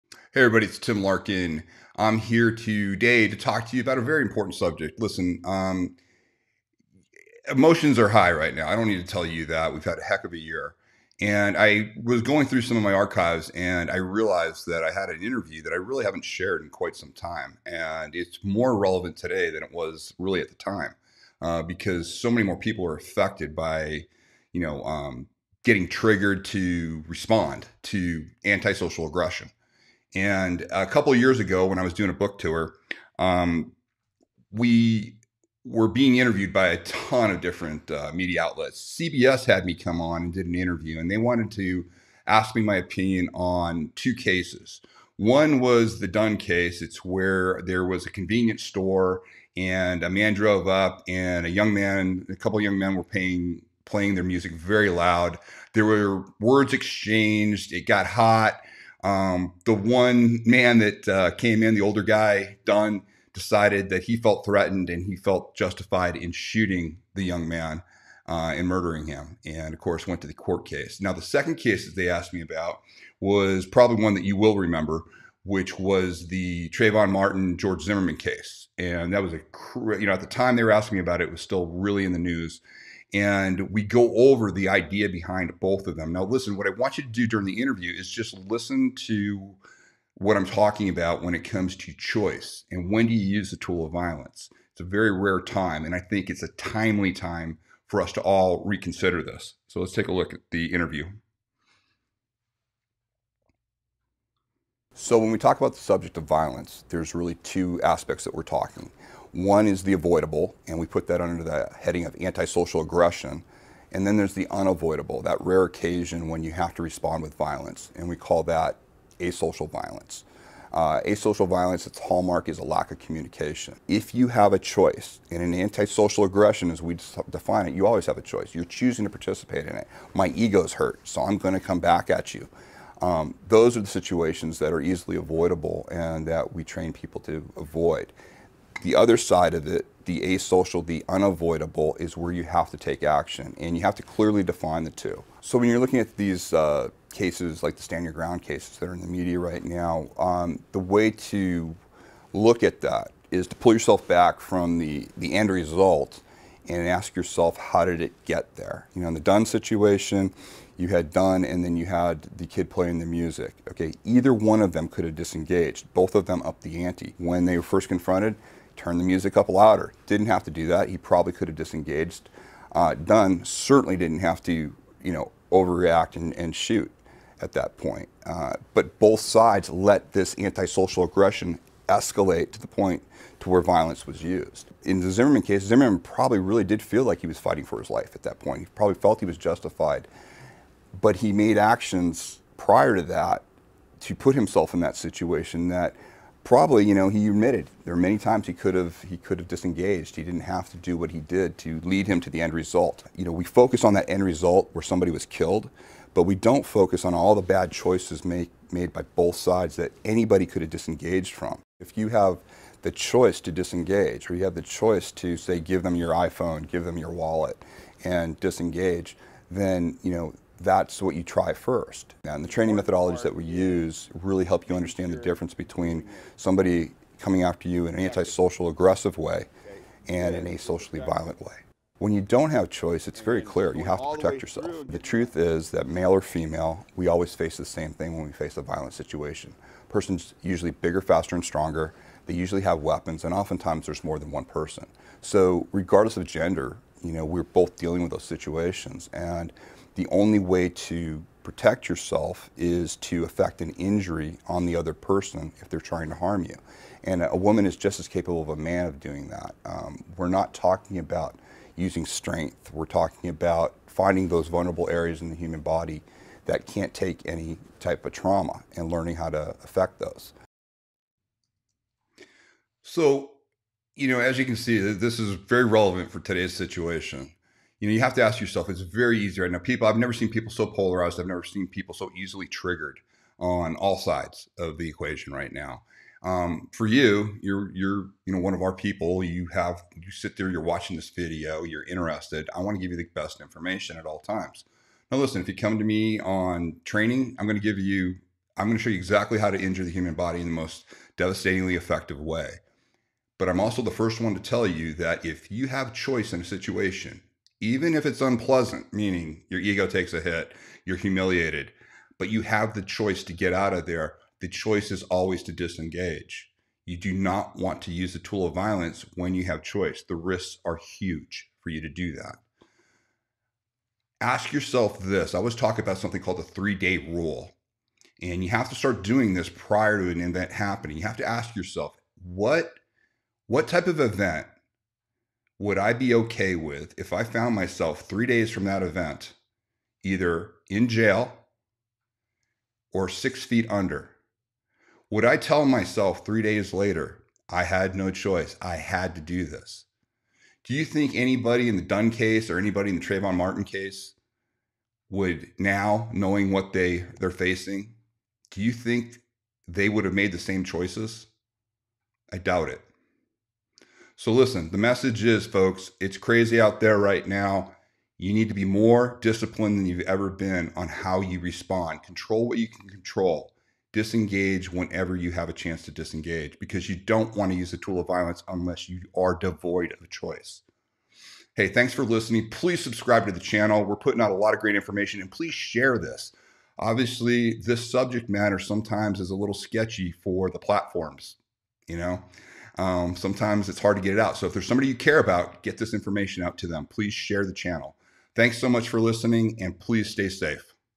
What To Do When You Have A Choice - Use Violence As A Tool Last - Tim Larkin - Target Focus Training
In today's video, I'm going to be showing you an interview with CBS. It covers the Dunn and Trayvon Martin/George Zimmerman case. We talk about the end result most often but too often ignore the steps both sides took to get there. We always have a choice, and using violence as a tool should be the last resort. I will explain the Three Day Rule, and how you always have a choice. This has never been more important, as our environment today is crazier and more stressed than it has ever been.

TIMELINES
00:00 Introduction
01:42 CBS Interview - Dunn, Trayvon Martin, & George Zimmerman
04:33 Back To Today's Situation
04:57 You Always Have A Choice
05:24 The Three Day Rule
06:03 It's Crazy Out There & Discipline More Important Than Ever
06:21 Closing & Please Subscribe